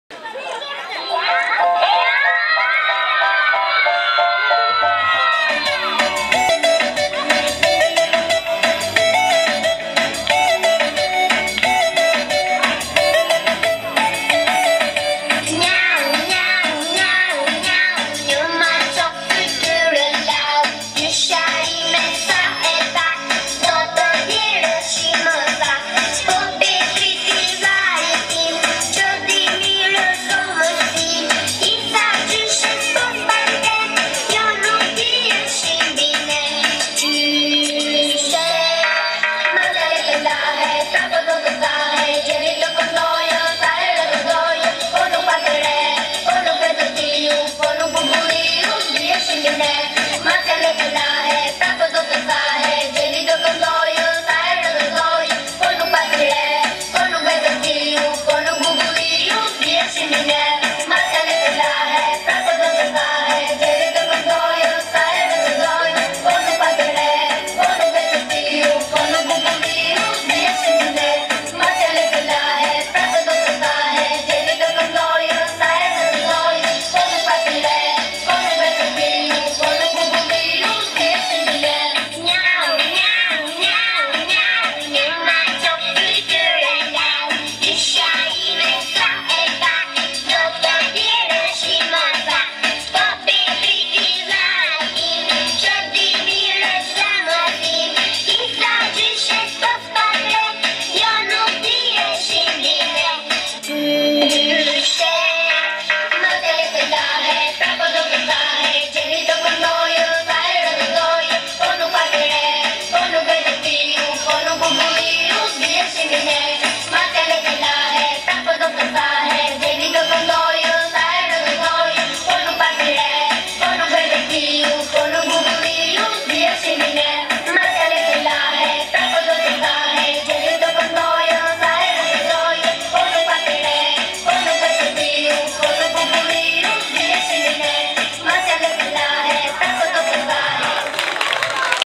Fragmente nga Festa e Abetares! Shkolla 9-vjeçare "Hidër Koka" - Xhafzotaj, Shijak
Gëzuar Festën e Abetares!
Shkolla 9-vjeçare "Hidër Koka" - Xhafzotaj, Shijak
Dr. znj. Zana Koni Lila
Nën/dr. znj. Marjana Lamka
Mësuese znj. Lina Hoxha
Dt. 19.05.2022